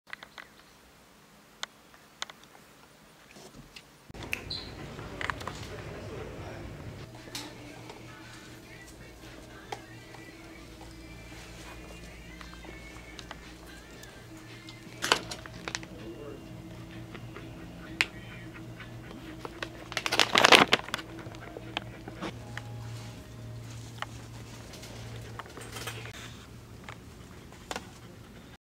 Dogs Birthday Outing
Duchess turned 7 ! Her birthday outing included going to the vet for a check up (don't think that was on top of her list to do but they gave her treats and wished her a happy birthday) , then we went to PetSense to pick out a present and then to Tractor Supply where she picked out one of her favorite snacks, Milk Bones ! Duchess is a komondor that we keep clipped. please check out our facebook